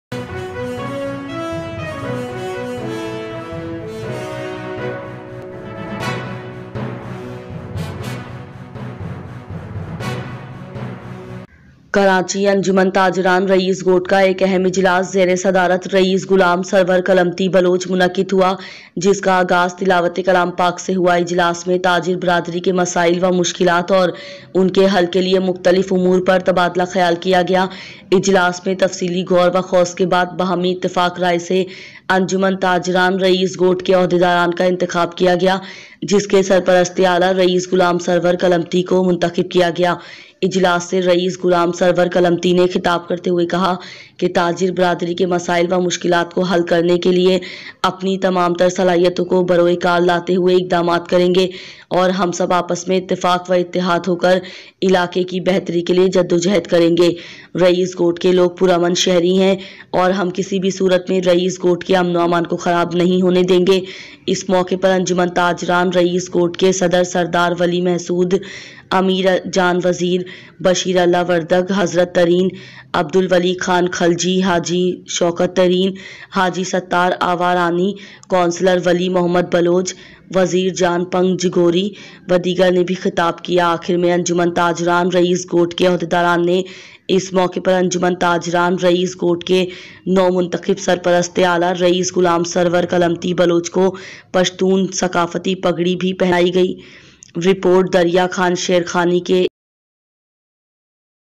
Karachi Anjuman-e-Tajiran Raees Goth was held under chairmanship Raees Ghulam Sarwar Kalmati Baloch
An important meeting of Karachi Anjuman-e-Tajiran Raees Goth was held under the chairmanship of Raees Ghulam Sarwar Kalmati Baloch

کراچی انجمن تاجران رئیس گوٹھ کا ایک اہم اجلاس زیر صدارت رئیس غلام سرور کلمتی بلوچ منعقد ہوا جس کا آغاز تلاوت کلام پاک سے ہوا اجلاس میں تاجر برادری کے مسائل و مشکلات اور ان کے حل کیلئے مختلف امور پر تبادلہ خیال کیا گیا اجلاس میں تفصیلی غور و خوص کے بعد باہمی اتفاق رائے سے انجمن تاجران رئیس گوٹھ کے عہدیداران کا انتخاب کیا گیا جس کے سرپرست اعلیٰ رئیس غلام سرور کلمتی کو منتخب کیا گیا اجلاس سے رئیس غلام سرور کلمتی نے خطاب کرتے ہوئے کہا کہ تاجر برادری کے مسائل و مشکلات کو حل کرنے کیلئے اپنی تمام تر صلاحیتیوں کو بروئے کار لاتے ہوئے اقدامات کرینگے اور ہم سب آپس میں اتفاق و اتحاد ہو کر علاقے کے بہتری کیلئے جدوجہد کریں رئیس گوٹھ کے لوگ پر امن شہری ہیں اور ہم کسی بھی صورت میں رئیس گوٹھ کے امن و امان کو خراب ہونے نہیں دینگے اس موقع پر انجمن تاجران رئیس گوٹھ کے صدر سردار ولی محسود،امیر جان وزیر،بشیر اللہ وردگ،حضرت ترین، عبدالولی خان خلجی، حاجی، شوکت ترین، حاجی ستار آوارانی، کونسلر ولی محمد بلوچ، وزیر جان پنجگوری، و دیگر نے بھی خطاب کیا آخر میں انجمن تاجران رئیس گوٹھ کے عہدیداران نے اس موقع پر انجمن تاجران رئیس گوٹھ کے نو منتخب سرپرست اعلیٰ رئیس غلام سرور کلمتی بلوچ کو پشتون ثقافتی پگڑی بھی پہنائی گئی*
رپورٹ دریاخان شیرخانی کے ایچ آئی ڈسٹرکٹ کیماڑی رئیس گوٹھ